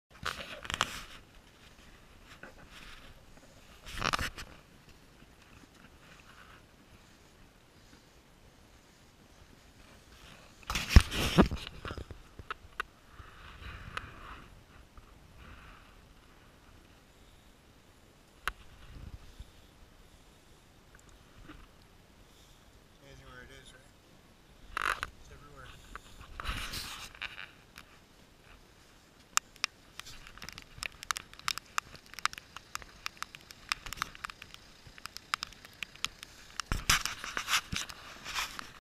MB Exit
Maison Blanche tunnel, near Vimy- Ridge, France. Video was taken 2019 by Tony Clarke